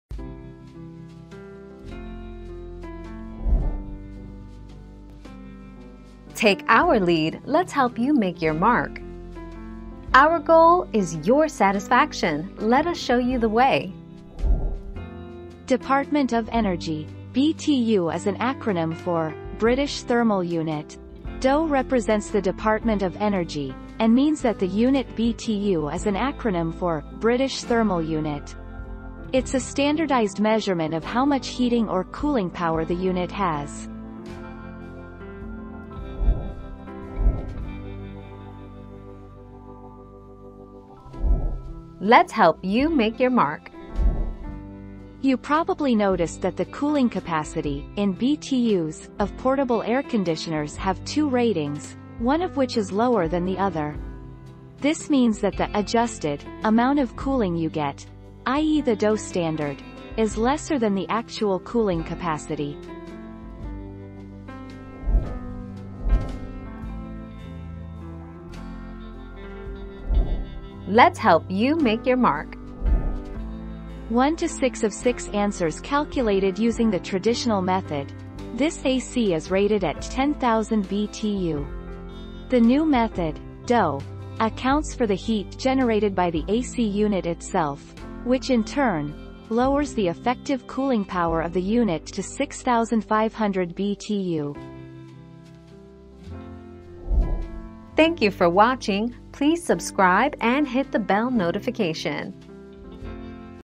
What does 5500 Btu DOE mean?
00:00 - What does 5500 Btu DOE mean?
00:38 - Why do air conditioners have 2 BTU?
01:06 - What does DOE mean after BTU?

Laura S. Harris (2021, July 5.) What does 5500 Btu DOE mean?
    AskAbout.video/articles/What-does-5500-Btu-DOE-mean-256971